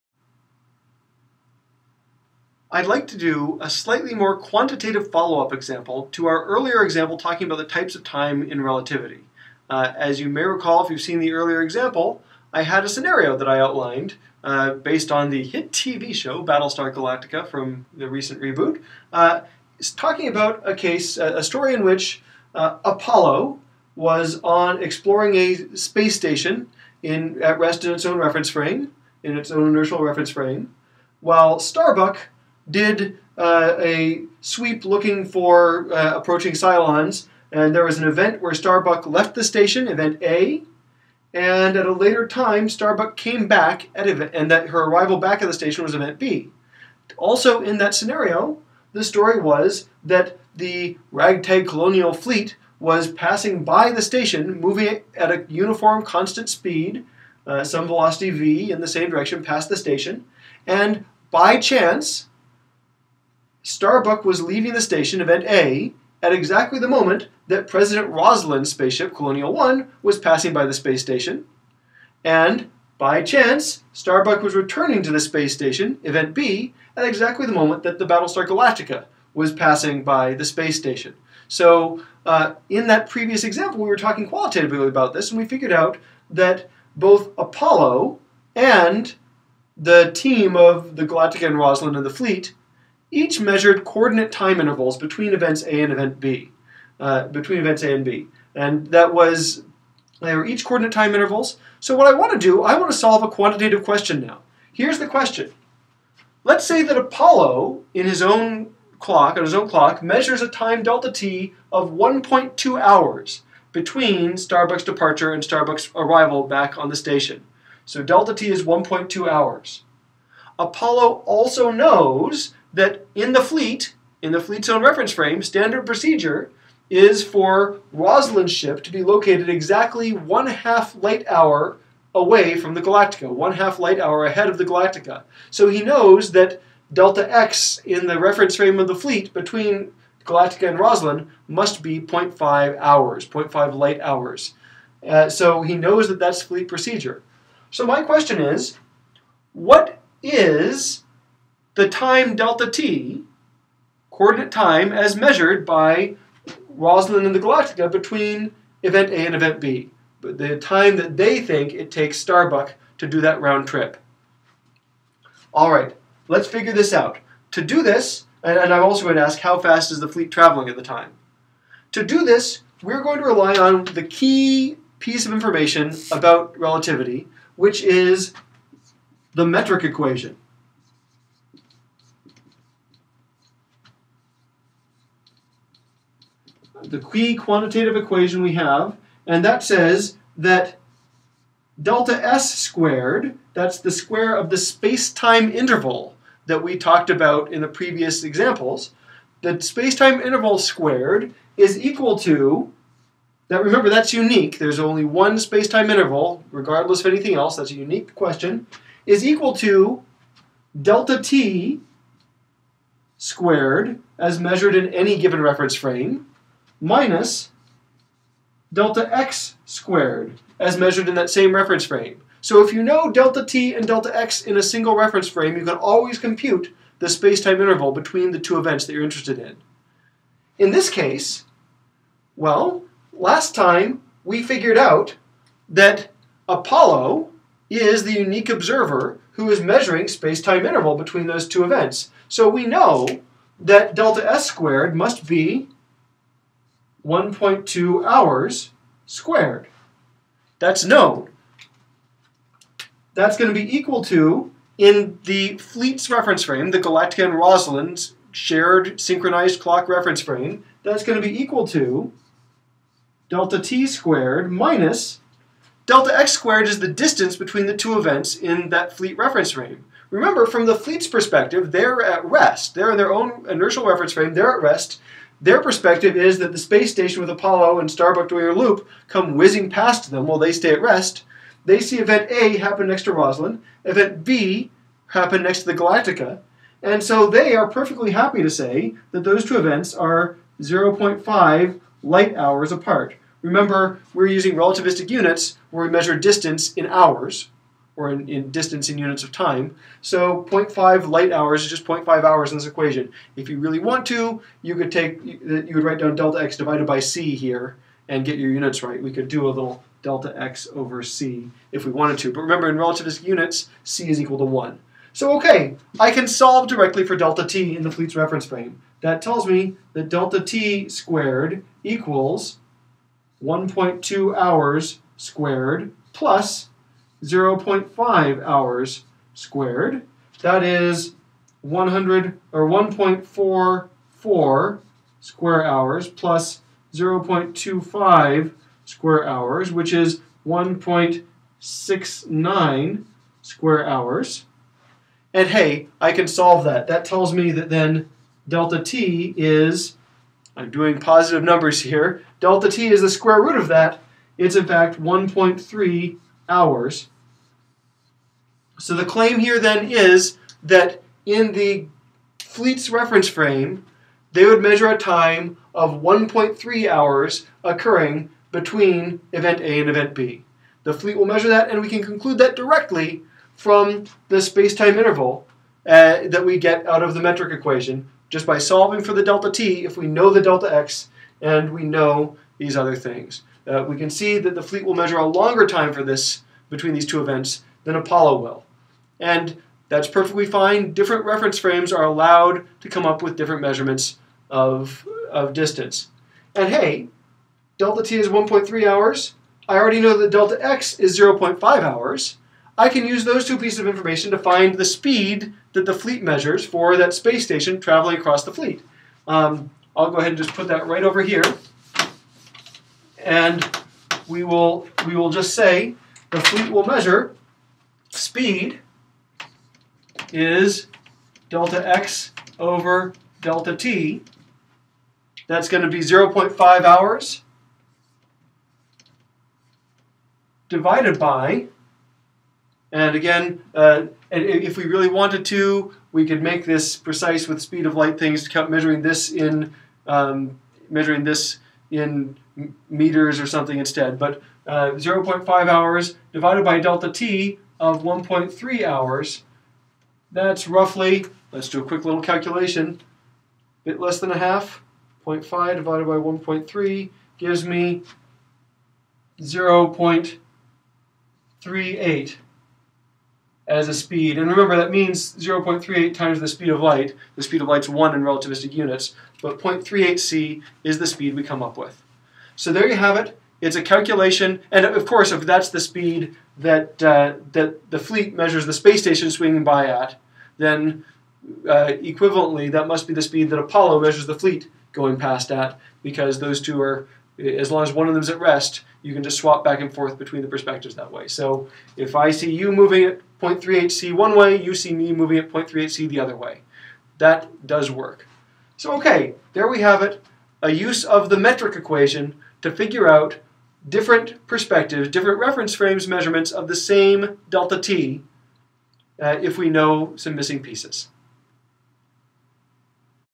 Relativity example: Using the metric equation
This example adds an actual computation to the scenario in an earlier video A followup video discusses how these results are sensible

Given known information about the time and space separation of two events in one reference frame, and a known spatial separation in a second frame, we can use the metric equation to compute the time separation in that frame.